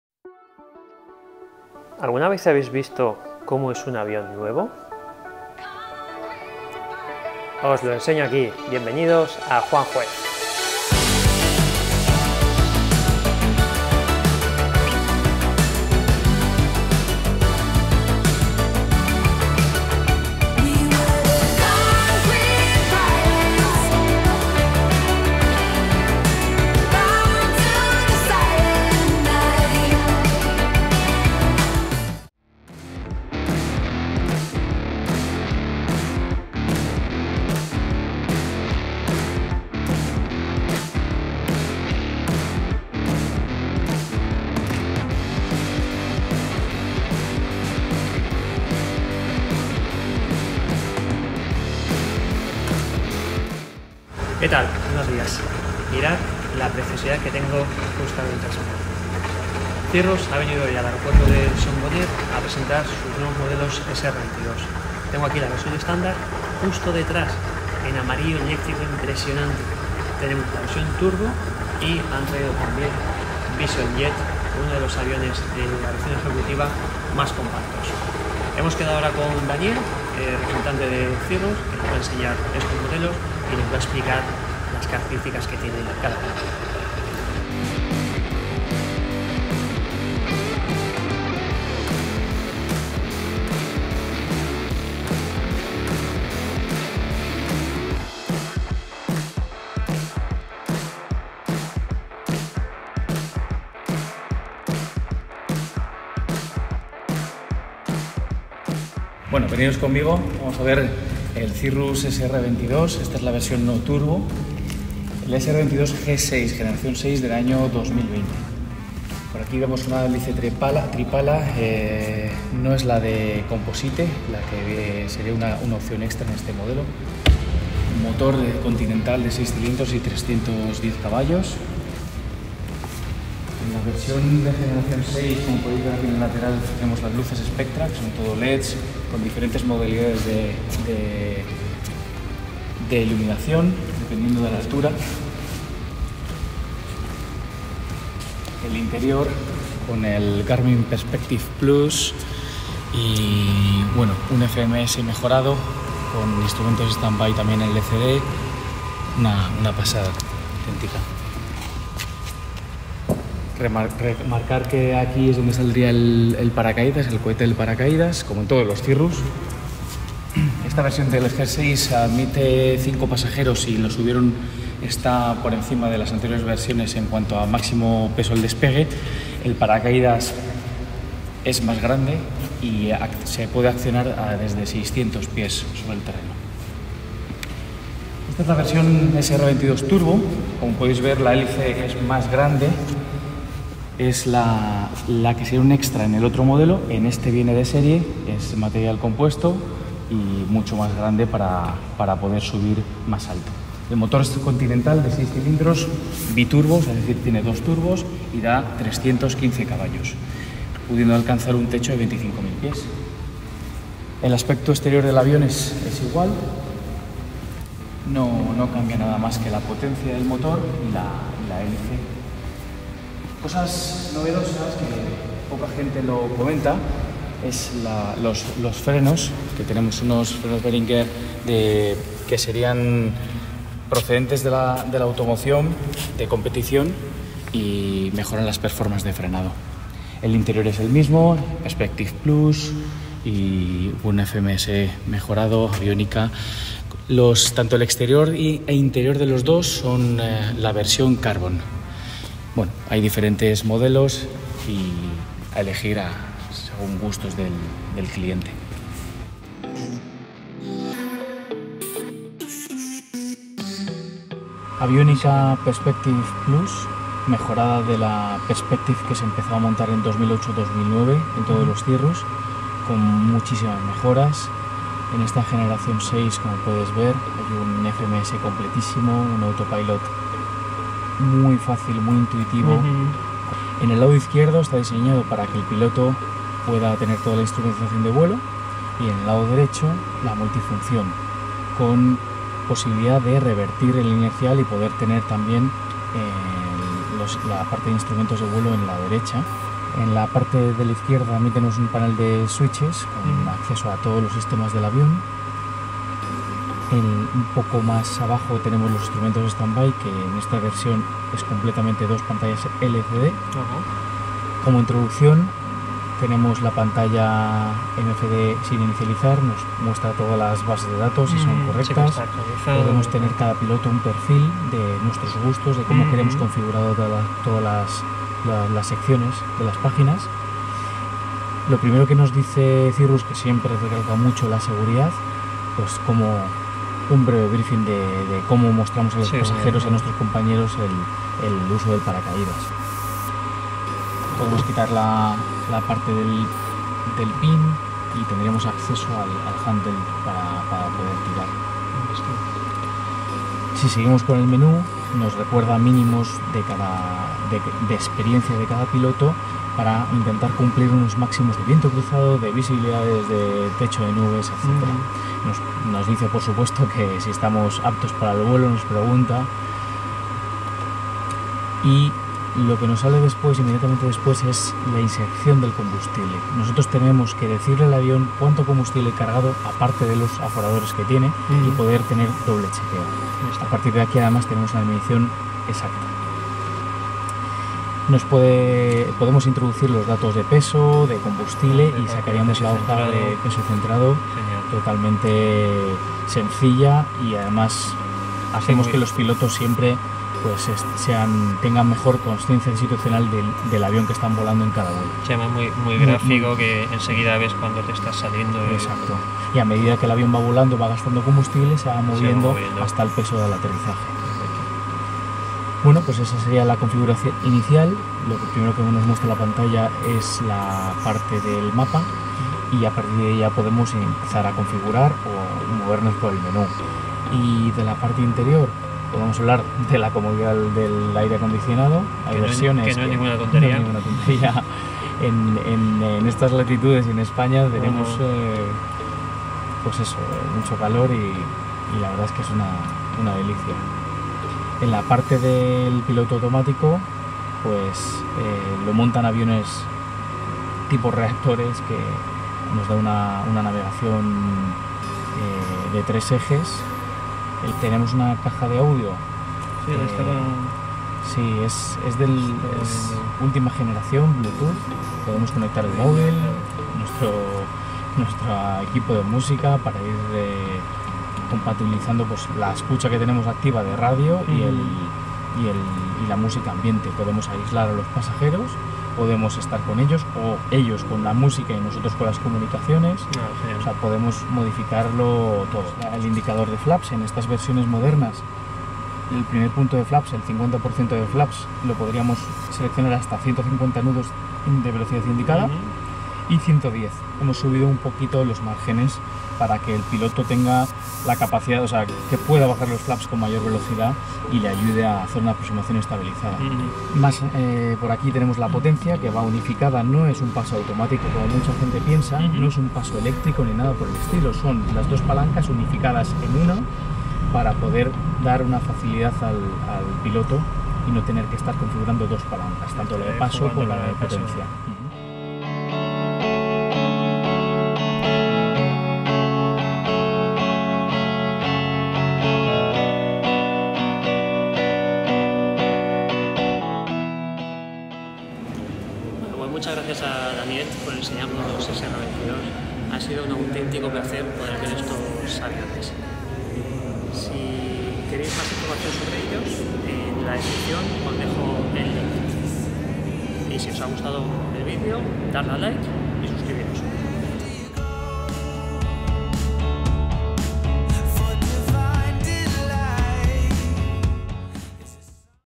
Descubre los NUEVOS Cirrus SR22 y SR22T G6 ✨ | ¿El Mejor AVIÓN Privado? 🤔
CIRRUS Spain me enseña los nuevos SR22 GTS, SR22T GTS G6 y Visión Jet.
Daniel, representante de Cirrus en España y Portugal me enseñó las novedades en la última y más nueva generación de Cirrus, la G6 2020.

Si quieres más información sobre Cirrus en español,

00:00 Intro
01:48 SR22 características
03:07 SR22T VOLT color características
04:24 Avionica SR22T, interior y configuración
08:44. outro

Comenta abajo y resolveré TODAS tus dudas.

SI TE GUSTA DALE AL LIKE Y A LA CAMPANITA PARA VER LOS PROXIMOS VIDEOS.